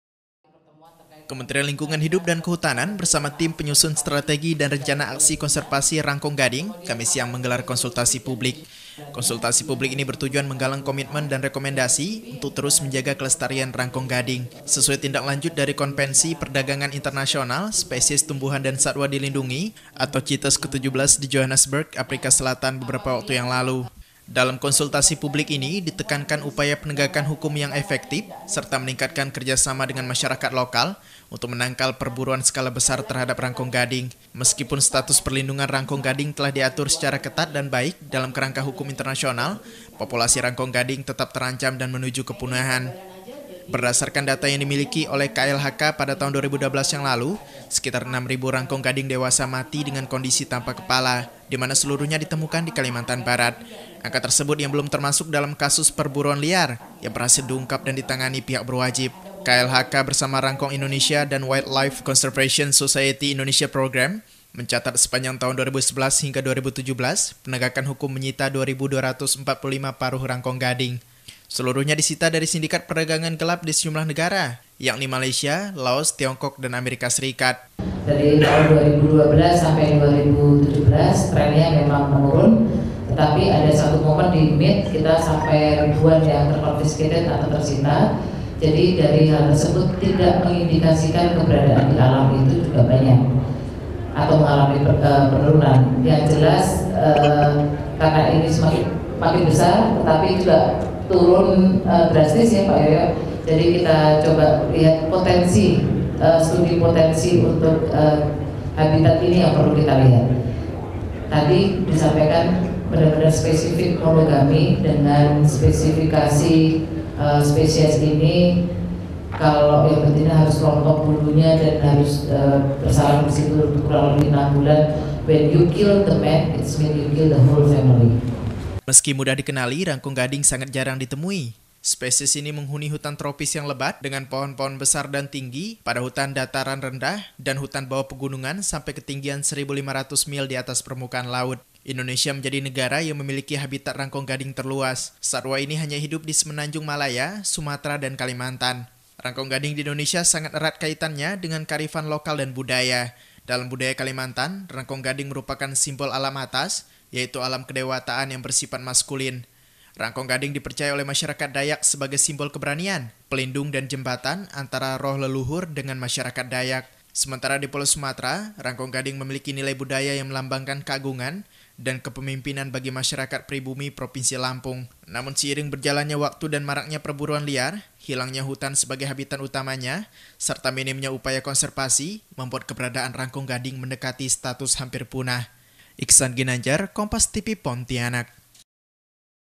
Pemerintah Susun Strategi Aksi Konservasi Rangkong Gading - KompasTV Pontianak
Kementerian Lingkungan Hidup dan Kehutanan, Kamis Siang menggelar Konsultasi Publik. Konsultasi Publik ini, bertujuan menyusun strategi mencegah kepunahan Burung Enggang Rangkong Gading, yang merupakan maskot Kalimantan Barat.